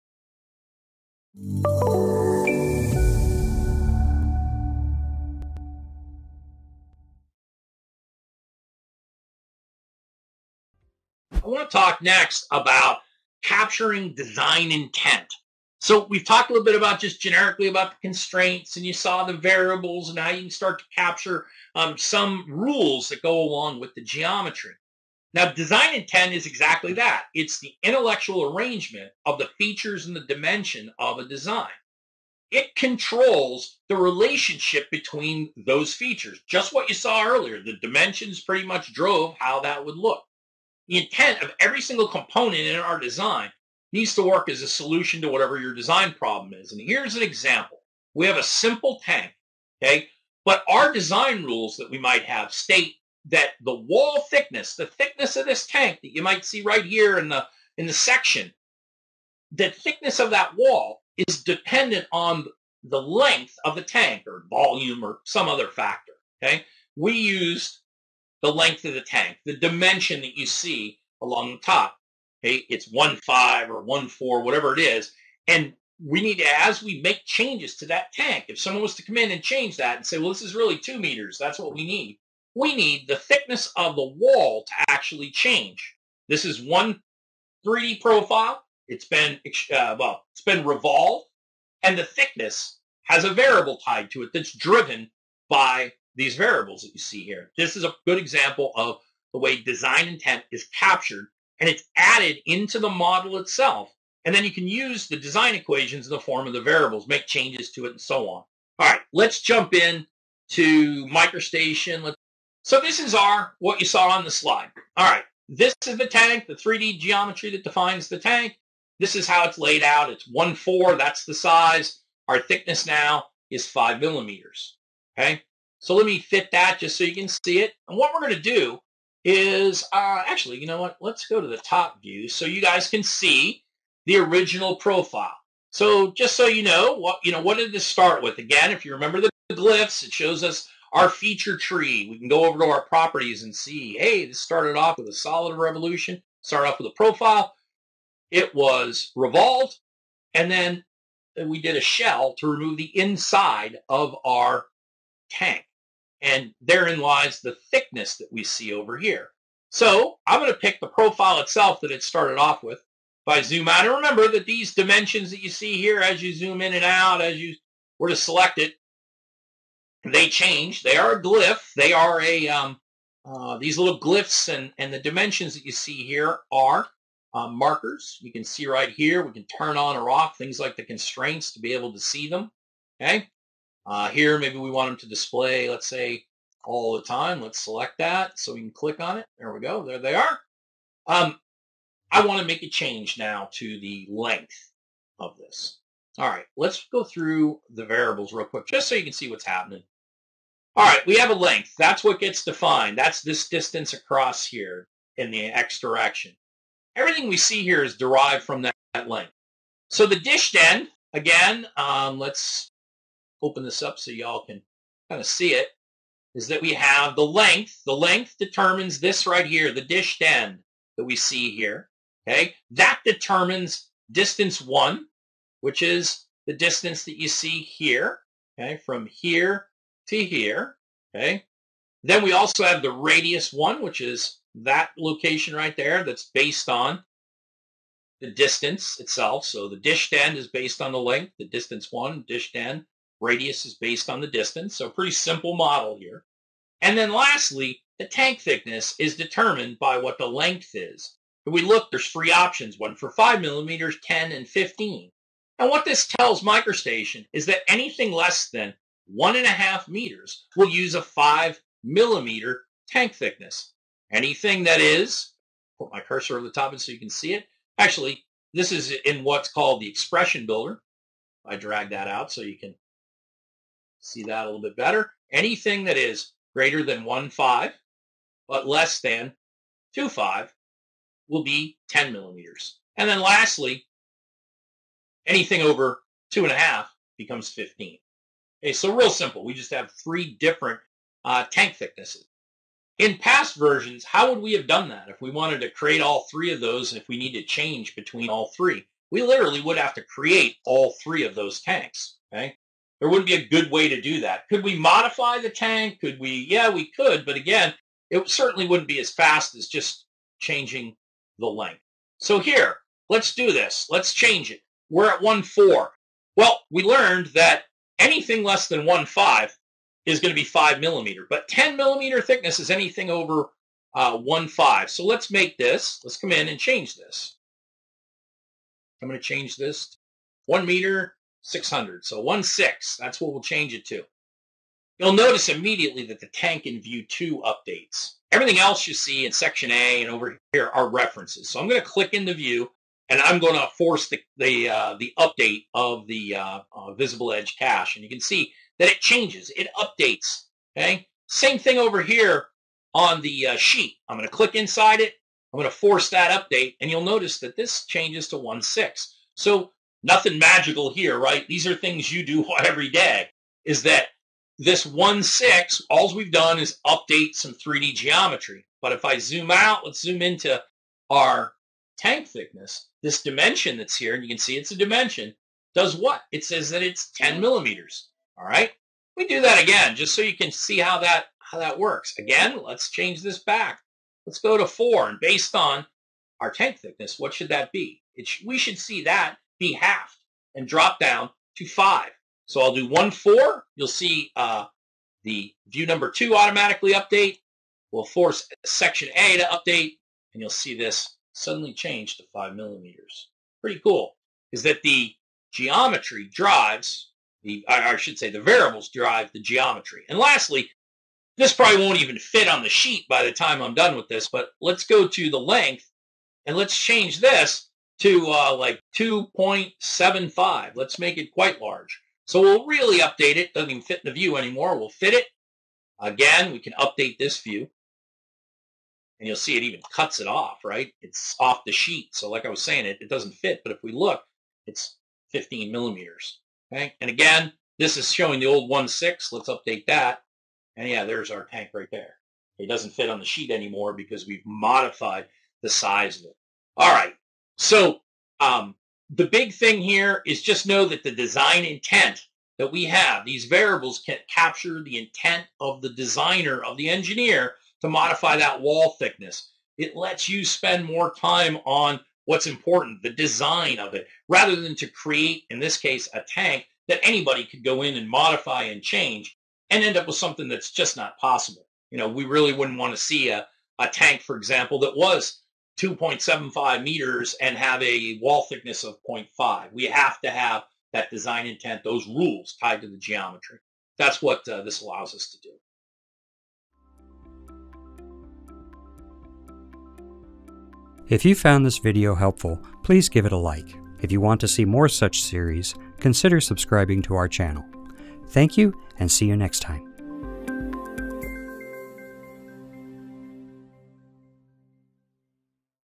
Part 3 Parametric Modeling: Design Intent
Learn how you can use MicroStation CONNECT Edition to create reusable models and cells that can be dynamically adjusted and sized with parametric dimensions and constraints.

This video covers Design Intent.